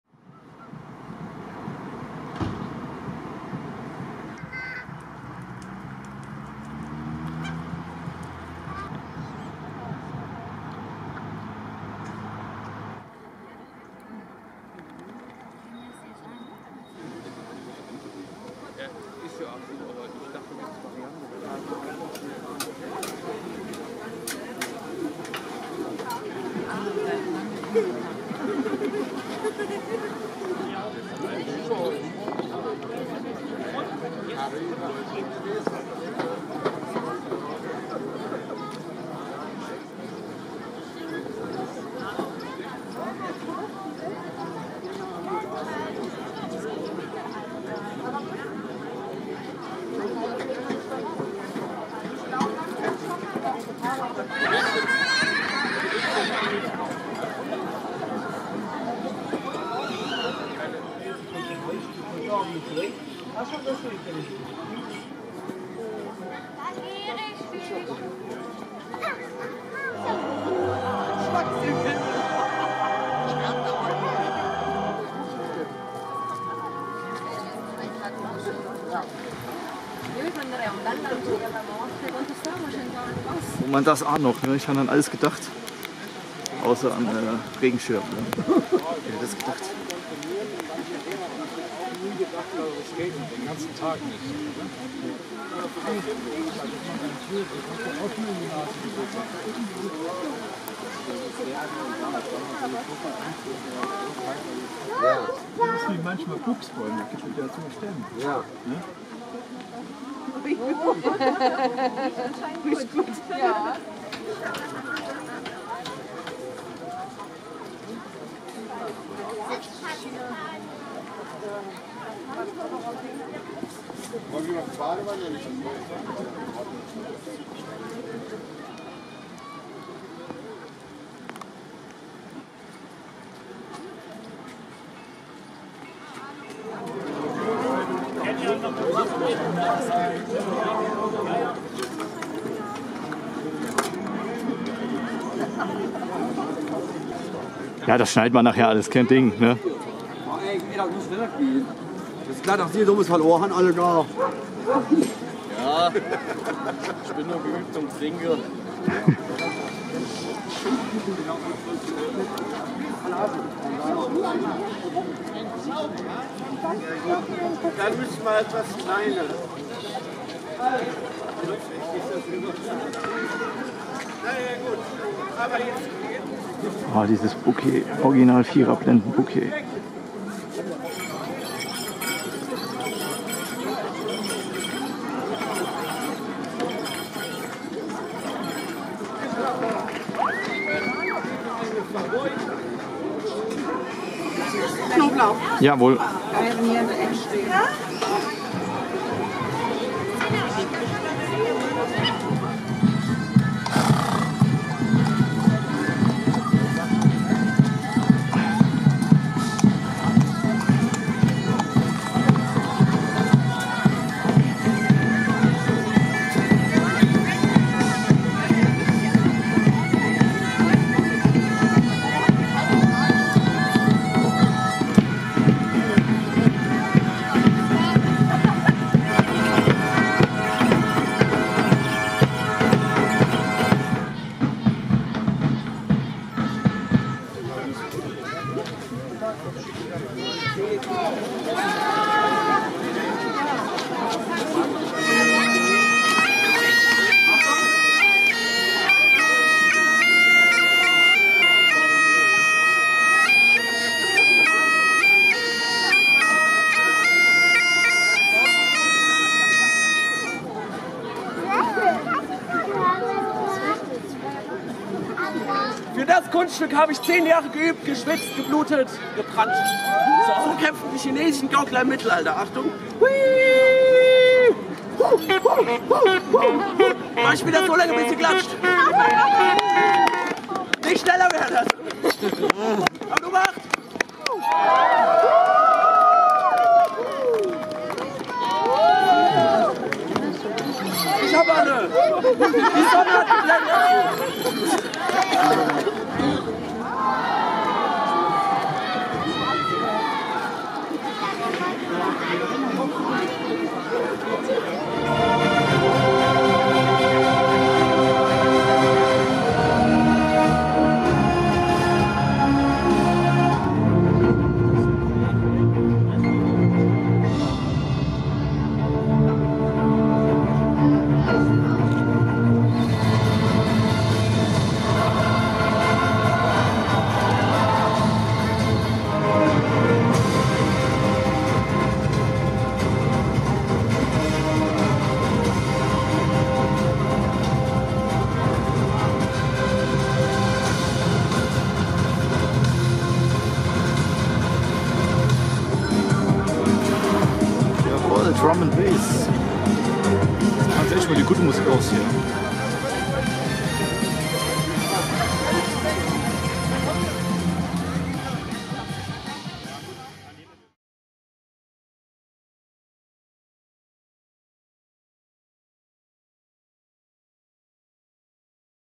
2. Mittelalter St. Martinsmarkt 2019 - DFG (u.a. mit Sidera-Fire feuershow)
Eindrücke vom 2. Mittelalter St. Martinsmarkt im DFG am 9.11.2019
u.A. mit Sidera-Fire